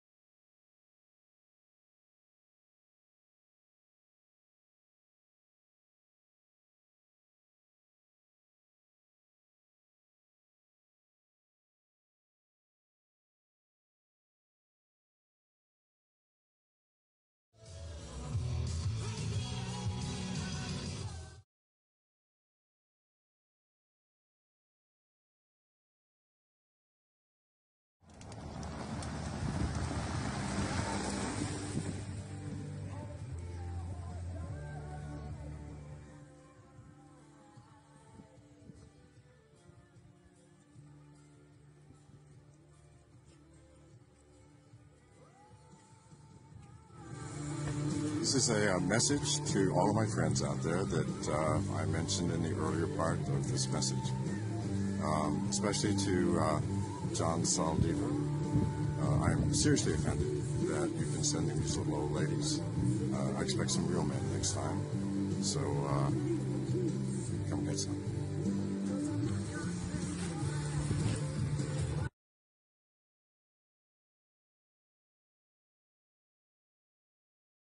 John McAfee’s Telling Video to John Saldivar
Remember John McAfee? He is the eccentric U.S. anti-virus software pioneer with a sordid history. Among other things, McAfee was a person of interest in the 2012 murder of his neighbor in San Pedro, another U.S. national, Gregory Faull.  In that same year, his property in Orange Walk was raided by the G.S.U.  McAfee claimed the raid was in retribution for his refusal to be extorted by the government.  He always claimed corruption in high places of government and always referenced Saldivar.  Those claims were dismissed. But after the Faull murder, he managed to elude the long arms of the law and fled to Guatemala where he was apprehended and eventually sent back to the U.S. where he continues to get attention. The year 2012 is featuring prominently in court proceedings in Utah for all sorts of things. And as the story involving John Saldivar picks up new legs, we found a telling online video by McAfee made years ago in 2014.  It is a private message to his friends, who he says, are involved in a treasure hunt.  What that means is difficult to tell, but among those he addresses in the video are: Minister John Saldivar, Lev Termendzhyan, who has Belizean nationality, and Gaspar Vega. McAfee ends the video directly addressing and taunting Saldivar.